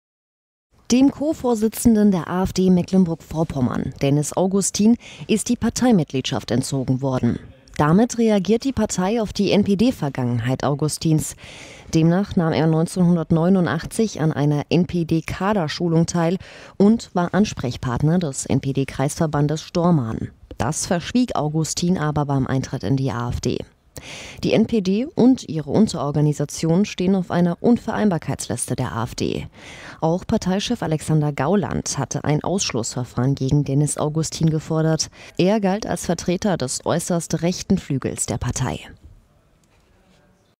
MECKLENBURG-VORPOMMERN: AfD entfernt Co-Chef Augustin aus der Partei
Der Machtkampf an der Spitze der Nordost-AfD ist entschieden: Der Landesvorstand wirft Dennis Augustin aus der Partei. Um dessen frühere NPD-Nähe sei es dabei aber nicht gegangen.